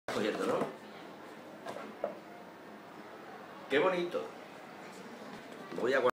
Manolo recibe en el Bar Correo los vasos de caña "Las Afueras".
Manolo, del Bar Correo, recibiendo los vasos de cerveza con la grabación de un verso de Las Afueras: que si te tengo algo de pan / o de ternura que prestarte.

En marzo 2012 se cumplían 15 años del poemario Las Afueras, de Pablo García Casado. Así que decidimos hacer un pequeño homenaje llevando los poemas a la calle. Por ejemplo, grabando uno de sus versos en los vasos de cerveza del Bar Correo de Córdoba. El poema completo:

me dices que la cama de tu cuarto
está sin hacer que bajaste y todas
las tiendas estaban cerradas que hoy

es domingo que ayer sábado dijimos
muchas cosas mucho amor ginebra besos
que si tengo algo de pan o de ternura

que prestarte

Las afueras, Pablo García Casado, DVD Ediciones, marzo 1997